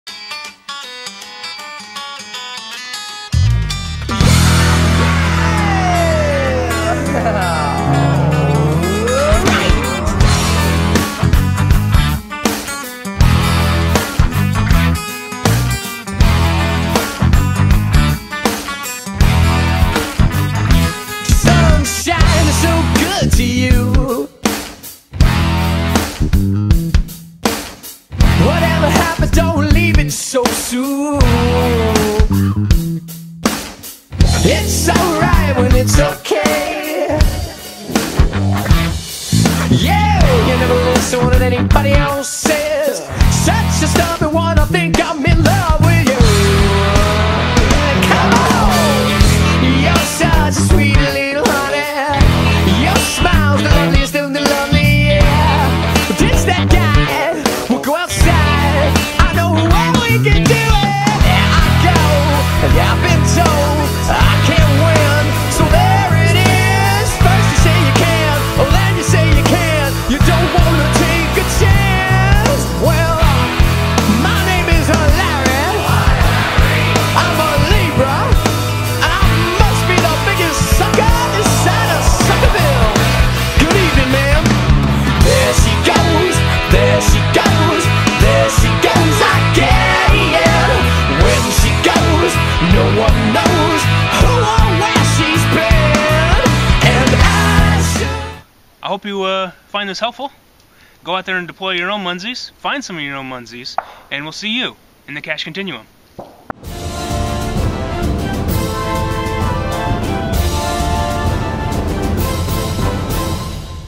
Munzee - Deploy & Capture
"Munzee is a real world scavenger hunt game where items are found in the real world and captured using your smartphone. You then level up and gain rank based on your score. Points are obtained by capturing other people's munzees or when your deployed munzees are captured by someone else. Munzee is based off of the fundamentals of geocaching and adds another layer of fun to the hunt."

Our entry for the contest Munzee is having about deploying and capturing Munzees.